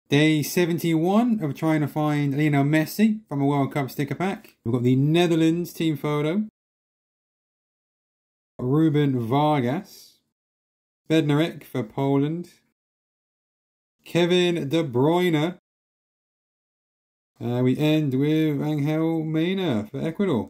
Opening a World Cup 2022 sticker pack every day until I find LIONEL MESSI! - Day 71
I STILL haven't found a Lionel Messi World Cup 2022 sticker. So I will be opening a Panini World Cup sticker pack every single day until I find him!